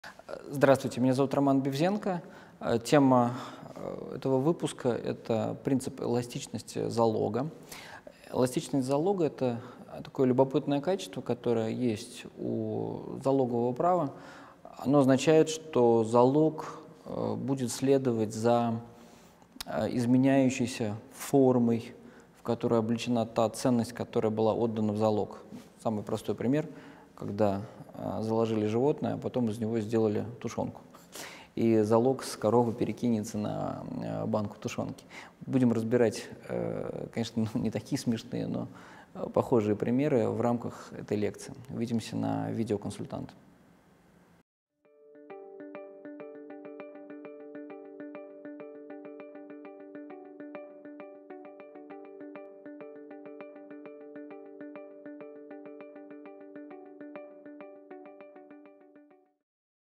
Смотрите на Видео.Консультант семинар «Случаи изменения предмета залога»
Тема лекции – принцип эластичности залога. Лектор раскроет понятие залога с практической точки зрения. Расскажет о механизмах замещения предмета залога, основываясь на выработанной судами практике.
Лектор – Бевзенко Роман Сергеевич, профессор Российской школы частного права при Правительстве РФ, член рабочей группы по подготовке реформы ГК РФ, партнер юридической компании «Пепеляев Групп», действительный советник юстиции РФ 2 класса, кандидат юридических наук.